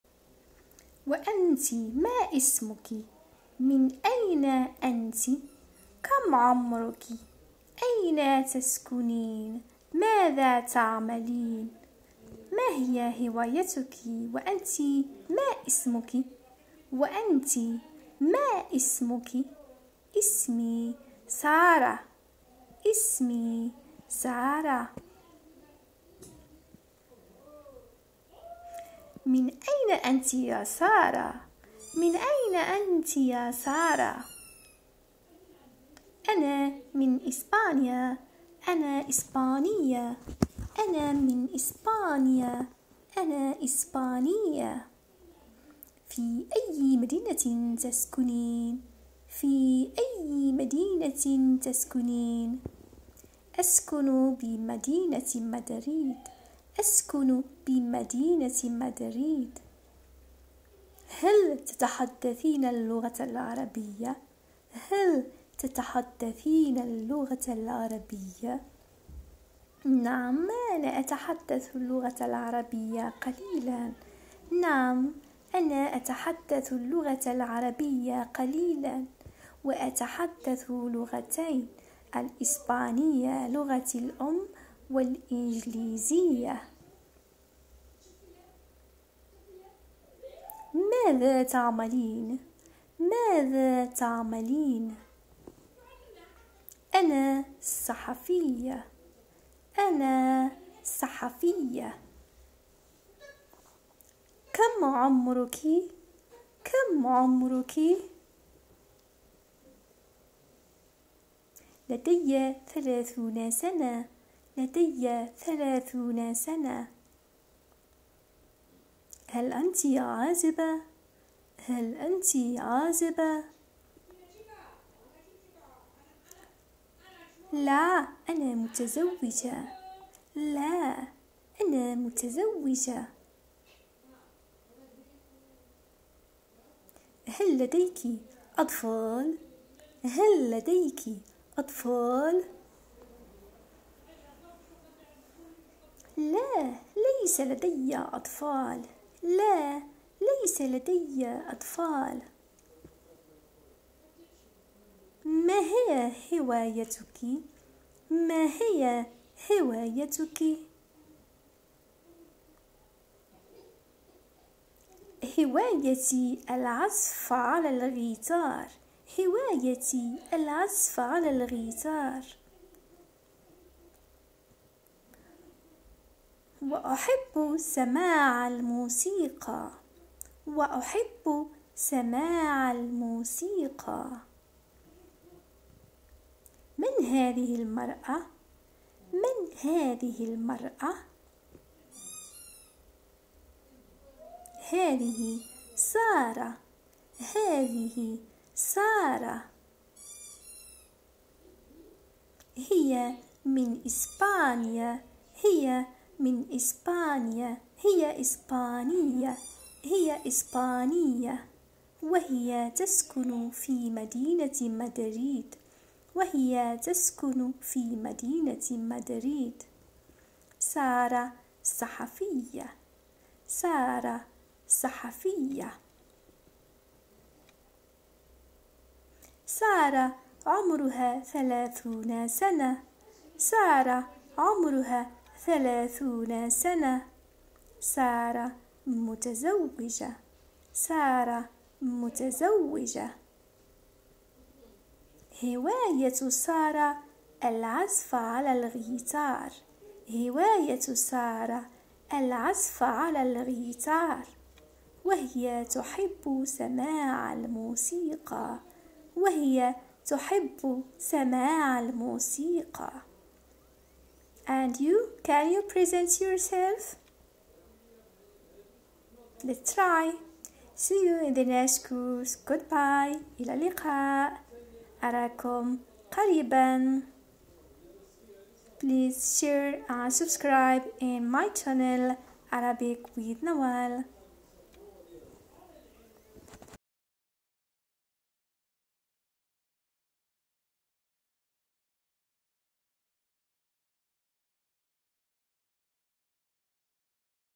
Short Arabic conversation/ self introduction Part2/level A1/مااسمك ؟ماذا تعمل؟ماهي هوايتك؟/
في هذا الحوار للمستوى  المبتدئ  A1  سيمكنكم الإجابة على أسئلة من نوع ما اسمك؟ كم عمرك؟ ماذا تعمل؟ أين تسكن؟ ما هي هوايتك؟ يمكنكم تبادل الأسئلة مع صديق او يمكنكم  تكرار العبارات مع أنفسكم باعتبارها صديق جيد أيضا،ولا تنسوا بأن الشغف هو الحافز الأول لكم على المثابرة والاستمرار في تعلم  اللغة التي تحبونها. حظا موفقا اصدقائي
In this short  dialogue for beginner level A1, you will be able to answer questions such as: What is your name? How old are you? What do you do? Where do you live? what is your Hoppy? You can exchange questions with a friend, or you can repeat the phrases to yourself as a good friend as well, and do not forget that passion is the first motivation for you to persevere and continue learning the language you love. good luck my freinds